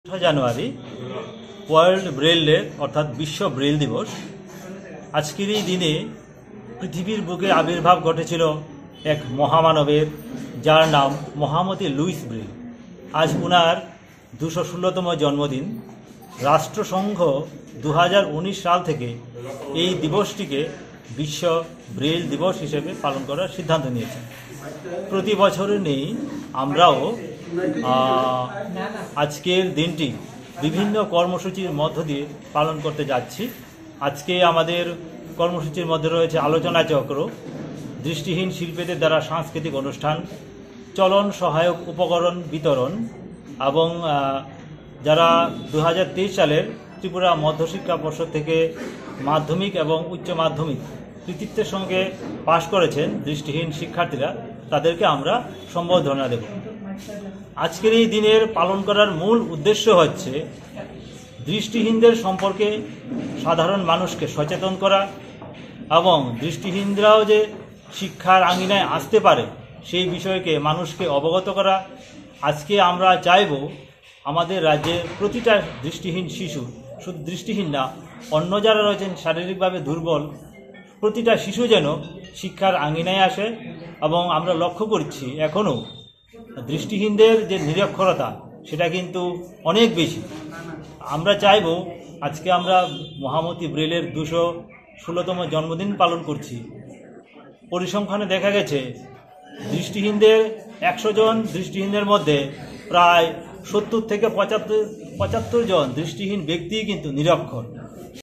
বৃহস্পতিবার বিশ্বে মহামতি লুই ব্রেইলের জন্ম হয়েছিল। আজ ওনার ২১৬ তম জন্মদিন।
বৃহস্পতিবার বিশ্বে মহামতি লুই ব্রেইলের জন্ম হয়েছিল। আজ ওনার ২১৬ তম জন্মদিন। বৃহস্পতিবার আগরতলায় প্রেস ক্লাবে দিনটি উদযাপন করা হয়।